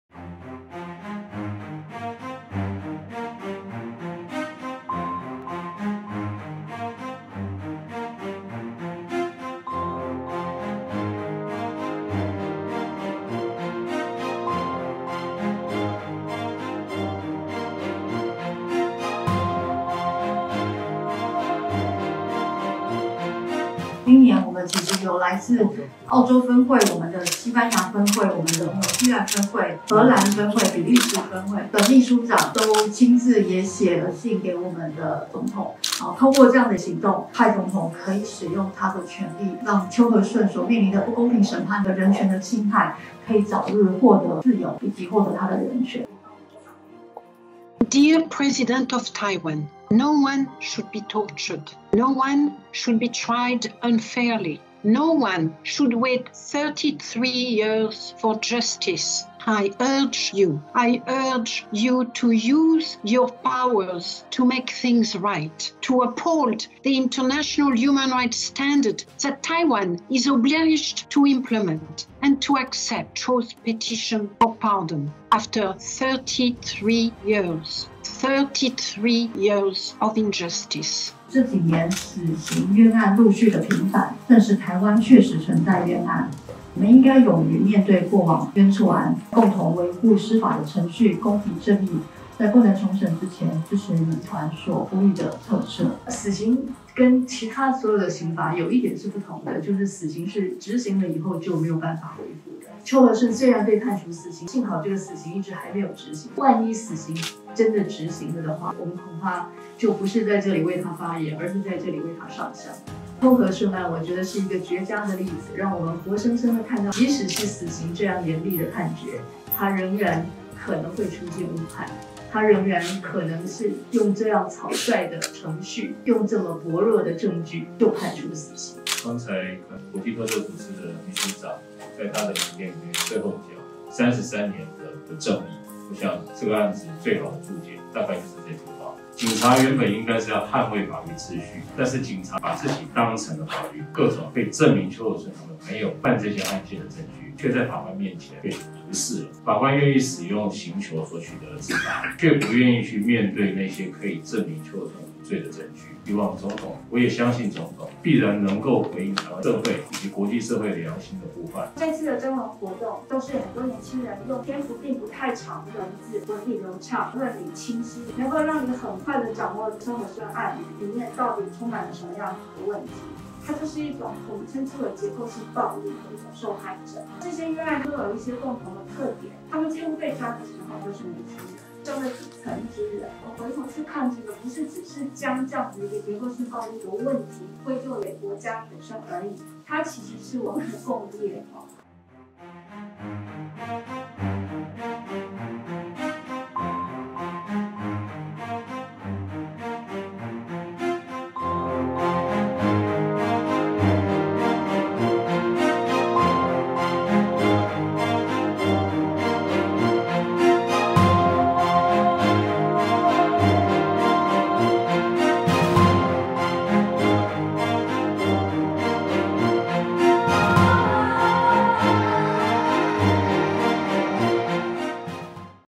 【台灣】遞交全球的邱和順徵文給總統 一起呼籲
【為邱和順寫一封信給蔡英文總統】遞交行動 📮

國際特赦組織於去年舉辦 為邱和順寫一封信給蔡英文總統 徵文活動，蒐集來自國內外的人權支持者為邱和順寫下一封信，請求蔡總統特赦邱和順。投稿者來自各行各業，學生、志工、家庭主婦、上班族等，更有許多年輕人藉由文字的力量為邱和順發聲。如今國際特赦組織遞交民眾所寫的數十封中英文信件、數萬個連署，由國際特赦組織秘書長阿格妮斯．卡拉馬爾親自發言，並邀請本次徵文活動的評審進行說明，將國內乃至國際不斷要求特赦邱和順的聲音送入總統府，盼總統能聽到人民的聲音。

國際特赦組織為邱和順案行動數十年，除與各社團奔走救援，更將邱和順選為 2012寫信馬拉松 全球倡議行動個案，讓全球的人權支持者為邱和順聲援、寫信；多年來國際聲援的訊息從不間斷，噓寒問暖、年節祝賀等訊息不斷地從世界各個角落捎進土城看守所的狹小牢房裡。

2020年，邱和順60歲，我們為再度他發起了全球連署及寫信行動——如今全球已收集超過42,700次行動——而甫進入2022年，更有來自國際特赦組織澳洲、西班牙、紐西蘭、荷蘭、比利時、葡萄牙、美國、德國等分會的秘書長，加上徵文活動的國內外民眾，親自寫信給蔡英文總統。

國際特赦組織與全世界超過40,000名支持者，呼籲蔡總統用其權力來處理對邱和順公正審判權的侵害，並接受赦免邱和順的請願、還給絕望的他遲來33年的自由與人權。